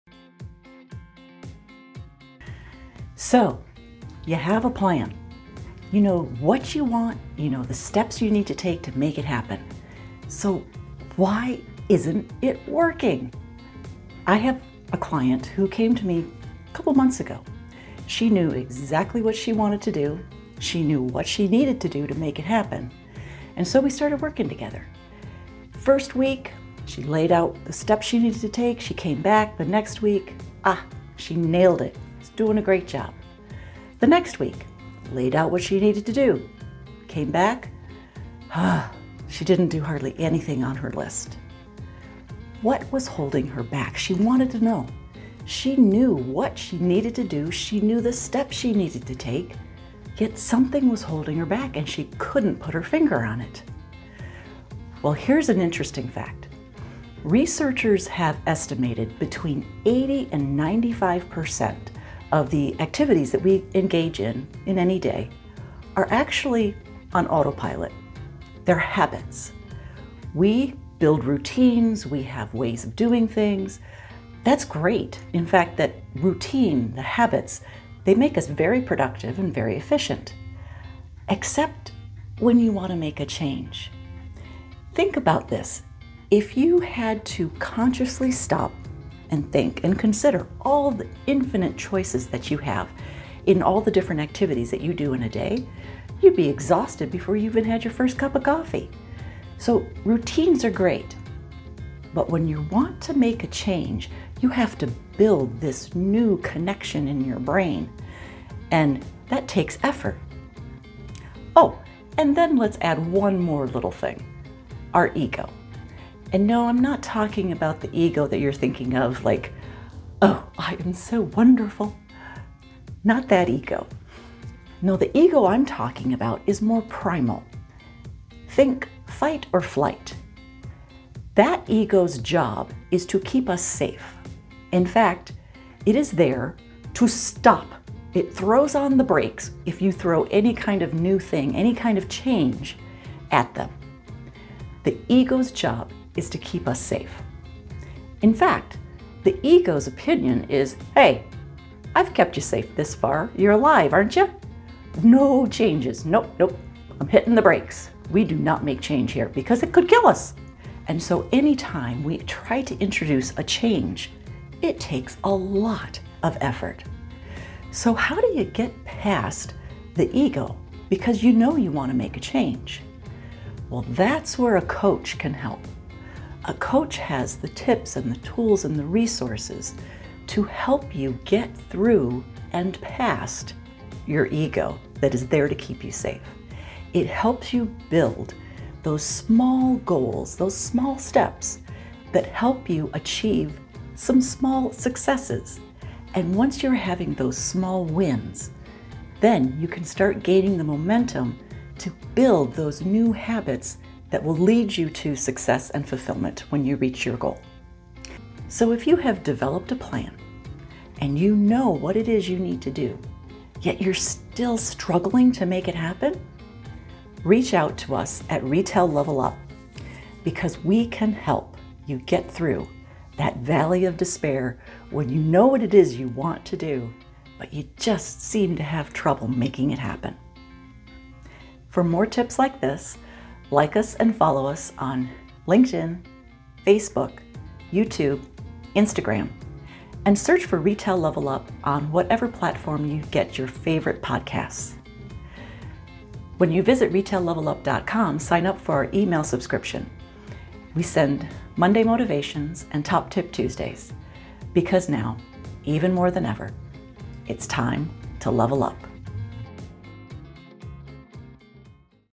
The Secret to Making a Change
You may know what want, and you may even know the steps you need to take to achieve your goal, but for some reason, you just can't seem to make it happen. Why is that?

It's because change is hard. We don't set out to sabotage ourselves, but the way our brains are hardwired to avoid change, it can be very challenging to achieve anything new or different. Learn how to overcome this obstacle in this video.